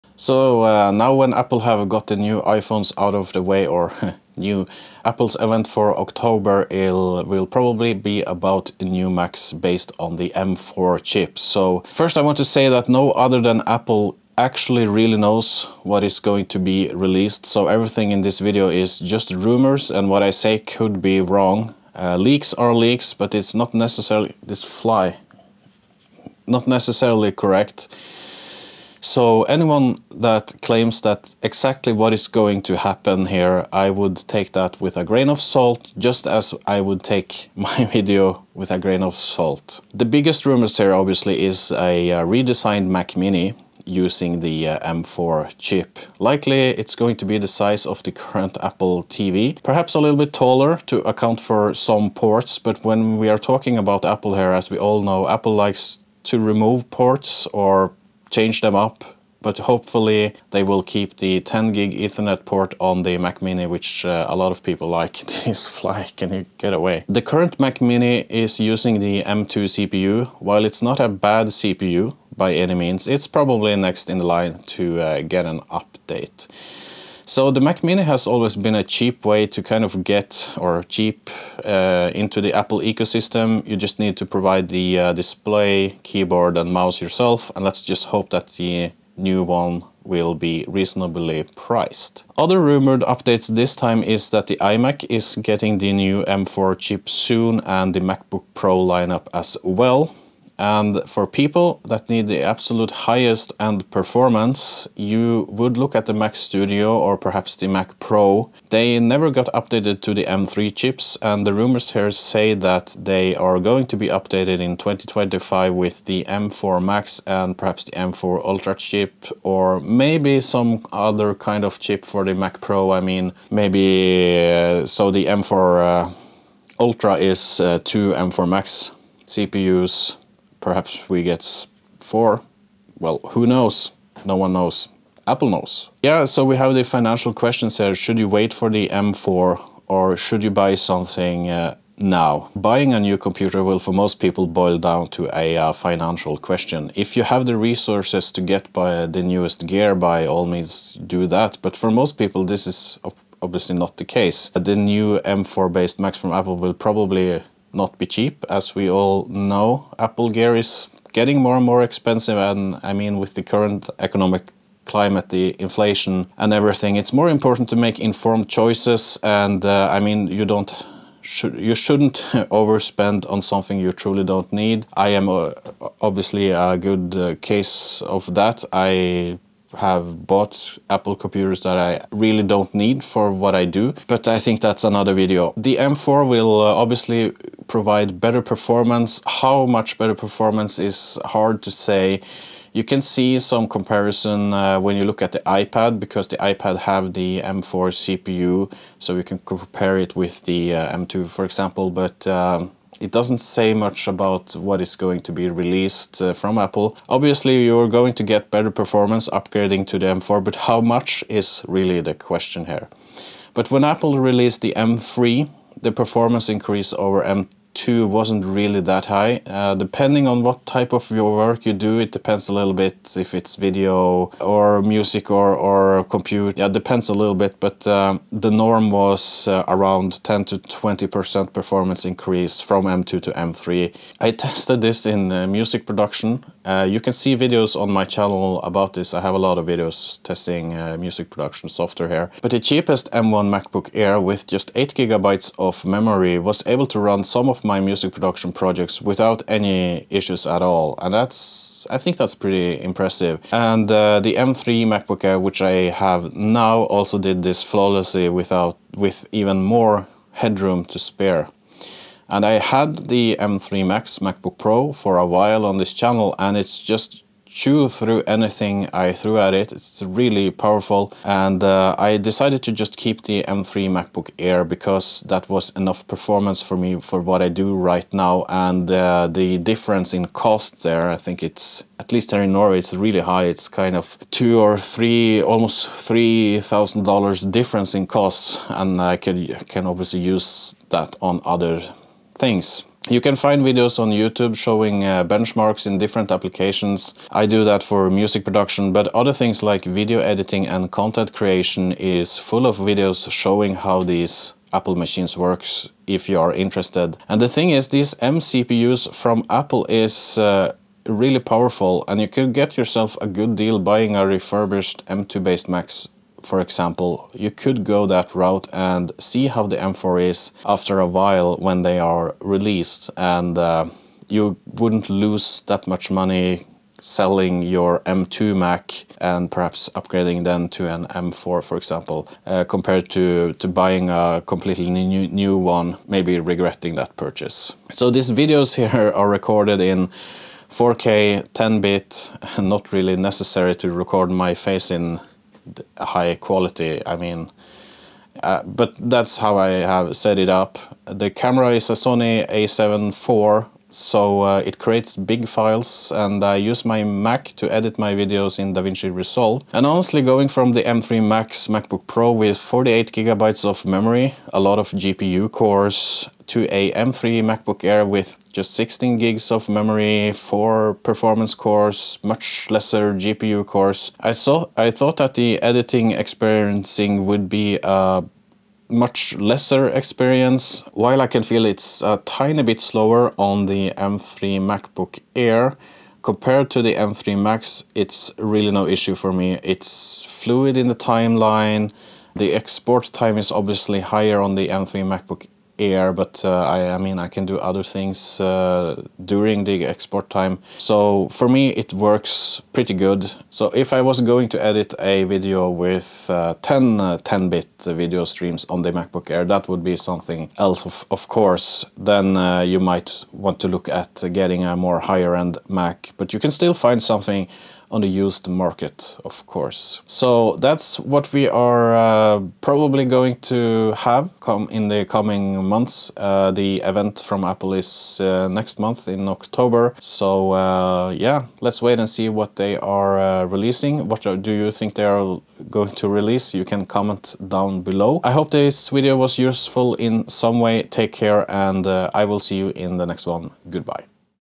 Wait for Apple M4 (new Mac Mini?) or Buy M1,M2,M3 Now
New M4 based macs are coming. What does the rumors say?

Main PC (AMD Ryzen)
Secondary PC (MacBook Pro)
Audio Interface (RME)
Headphones (DT 770 Pro)
Speaker Stands